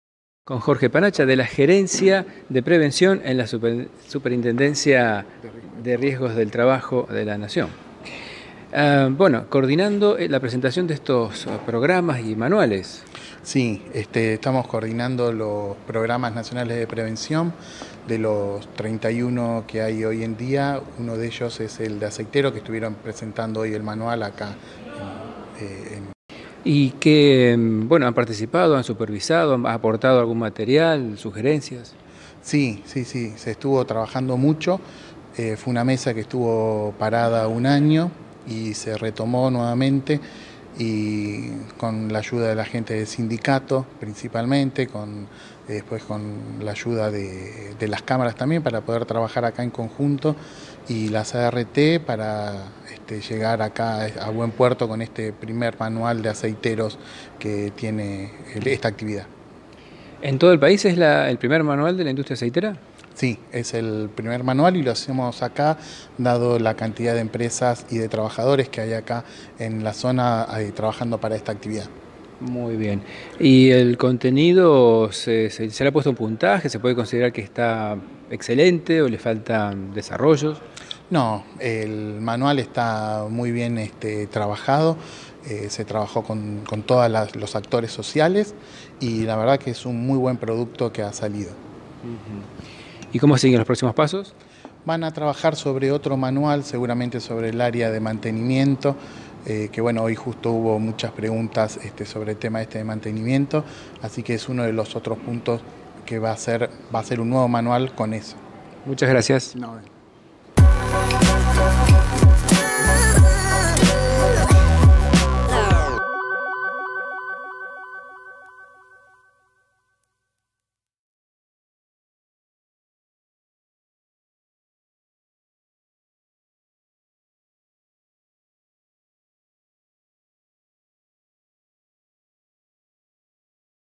JORGE PANACCIA EN PRESENTACION MANUAL BUENAS PRACTICAS INDUSTRIA ACEITERA
JORGE PANACCIA está en el Equipo de la Gerencia de Prevención de la Superintendencia de Riesgos del Trabajo de la Nación (SRT).

Desde el Programa Nacional de Prevención (PRONAPRE) coordina las presentaciones de Manuales de diversas industrias. El 21 de Diciembre 2018 se presentó en el Centro Cultural de PGSM el Manual de Buenas Prácticas de la Industria Aceitera, y se eligió esta ciudad por la concentración de empresas aceiteras y trabajadores de ese rubro.

El Manual fue desarrollado en conjunto entre la SRT, Cámaras, ART, el Centro Regional de Higiene y Seguridad de la CGT San Lorenzo, y el SOEA (Sindicato de Obreros y Empleados Aceiteros del Departamento San Lorenzo).

El SOEA tuvo una participación esencial, con varios Delegados y miembros de Comisión Directiva aportando conocimientos y experiencias directas para perfeccionar la prevención de accidentes.

Filmado en Puerto General San Martín, provincia de Santa Fe, Argentina, 21 Diciembre 2018

+Info:
Rosarinos.com, Sanlorencinos.com, Puertenses.com.ar
EncuentroDeColectividades.com, Colectividades.com.ar